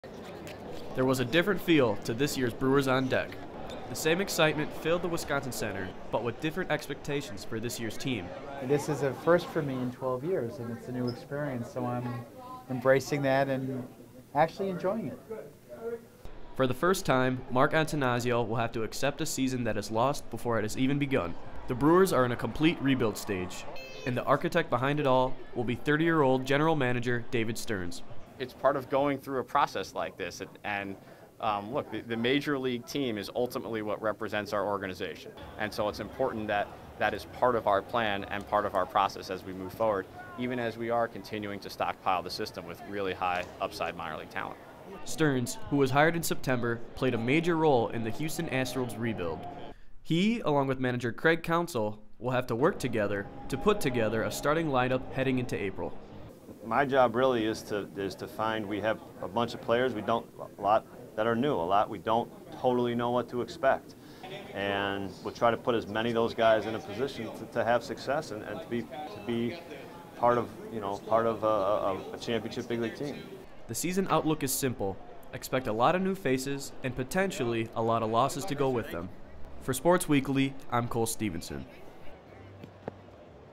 Brewers on Deck - Cole Stevenson
January 31, 2016 - The 2016 Milwaukee Brewers look forward to a question-filled season as they connect with fans.

Featuring: Mark Attanasio, David Stearns, and Craig Counsell.